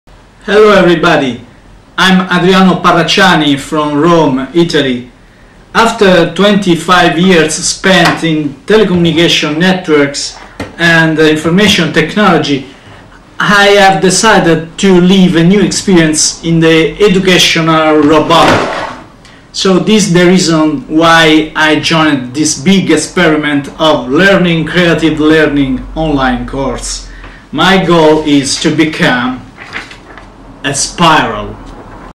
Adriano Parracciani Intro for Learning Creative Learning
my intro for MIT Media Lab course Learning Creative Learning